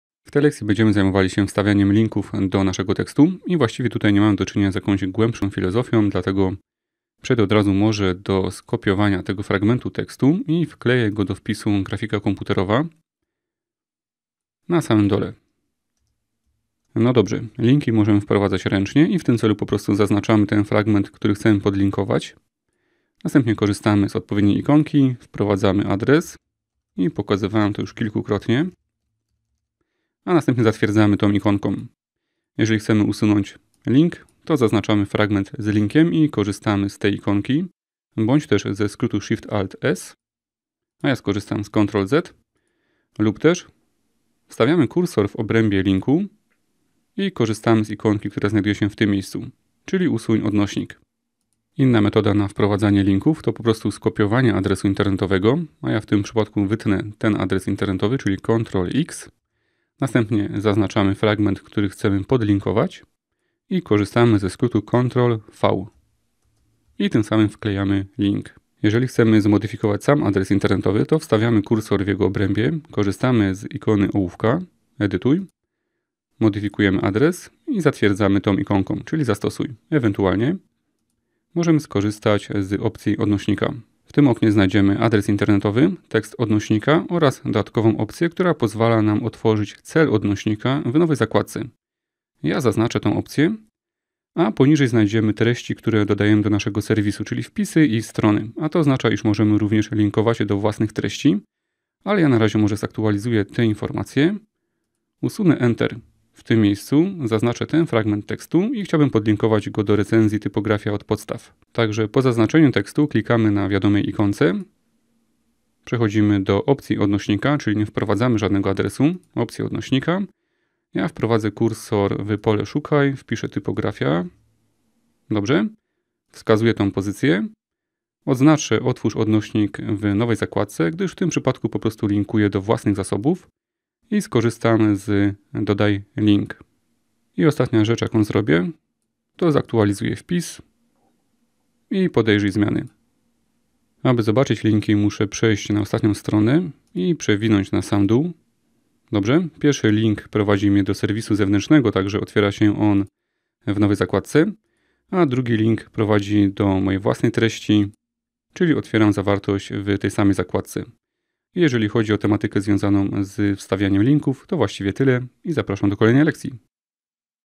WordPress - praca z treścią 18/42: Wstawianie odnośników
Szkolenie poświęcone pracy z treścią w WordPresie, od wpisów, poprzez media, kończąc na dodatkowych stronach.

Uwaga! Szkolenie oparte jest na WordPressie 4.8, niektóre informacje mogą być nieaktualne.

fot. H Heyerlein